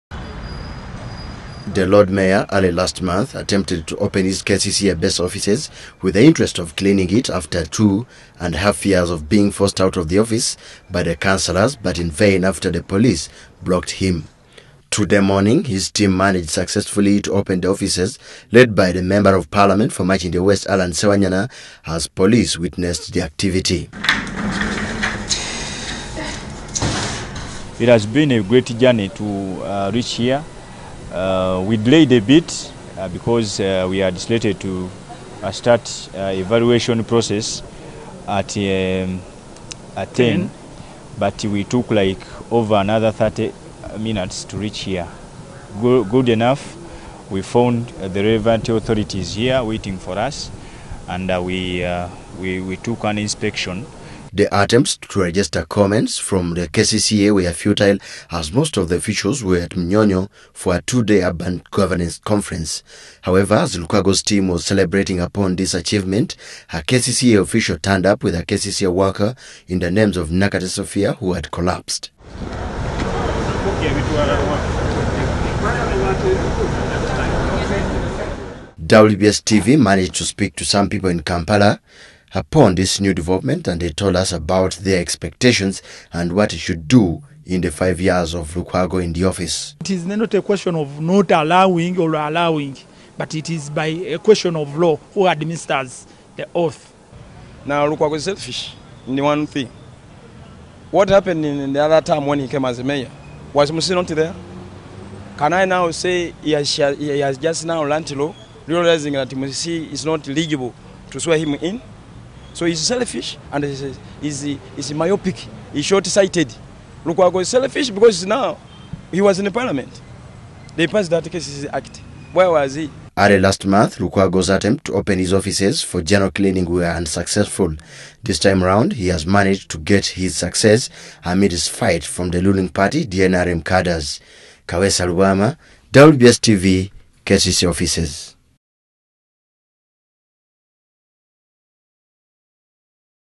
LORD MAYOR’S OFFICE OPENS
The Lord Mayor’s Parlour at City Hall has been opened as preparations for his swearing in on June 1st gather pace.
Getting to occupy the office has been a bone of contention between the embattled opposition politician and the KCCA Executive Director Jennifer Musisi.
Today’s opening of the Parlour came on the backdrop of concerns that Lukwago could again be blocked from accessing the premises in what has come to be one of the country’s political soap operas.